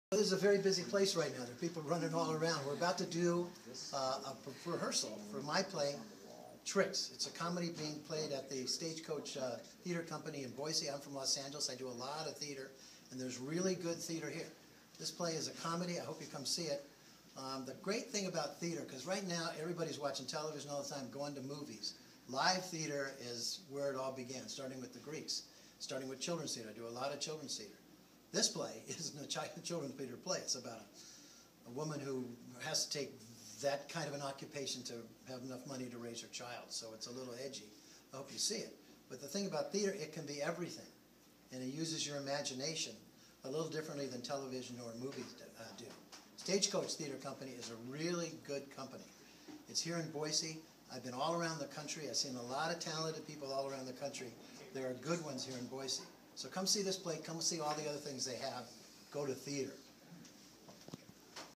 Lloyd Schwartz at Stage Coach Theatre
Famous tv producer/writer sharing his love for live community theatre and introducing the world premier of his play running at Stage Coach Theatre from July 12th - 27th.